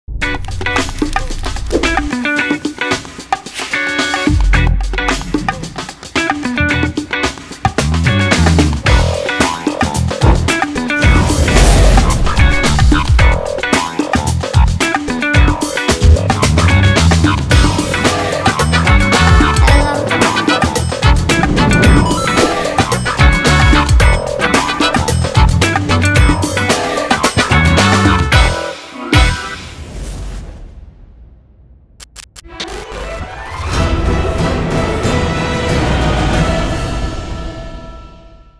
Endibo coconut yum-balls kokusz golyok bilute de cocos food commercial
Endibo coconut yum-balls - 2020
(bilute de cocos/ kokusz golyok)
Instinct Visuals 2020
Film si edit Produs de SC Instinct Films SRL - Tg Mures - 2020 05